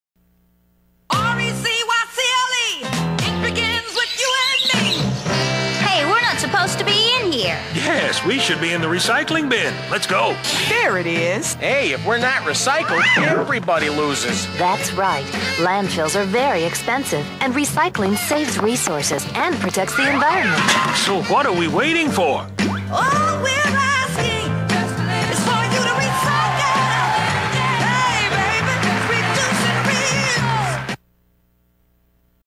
The Recycle Guys "Rollcart"
This video is the first in a series of Recycle Guys videos created to promote recycling in Indiana